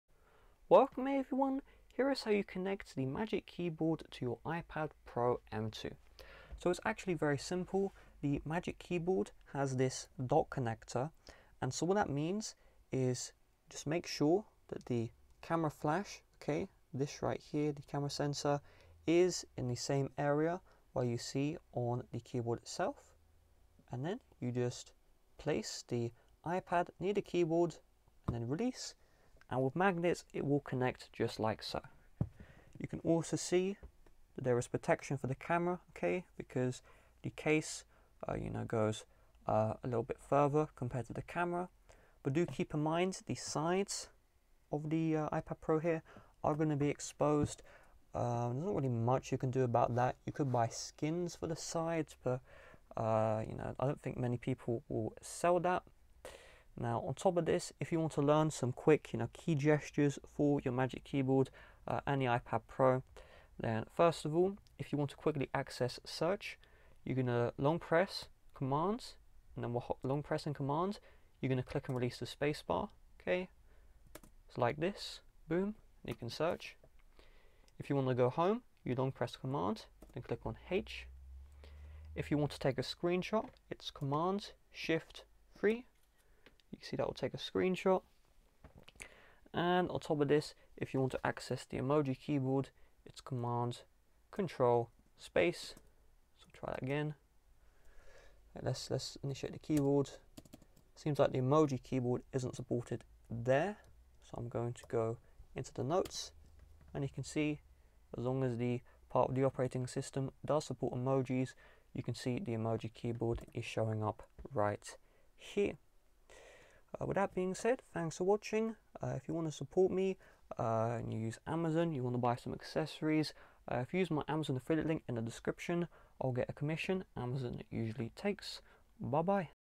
How To Connect Magic Keyboard to iPad Pro M2 (2022 Model)
Learn how to connect magic keyboard to iPad Pro m2 2022, and learn some quick actions from the keyboard for your iPad Pro as well.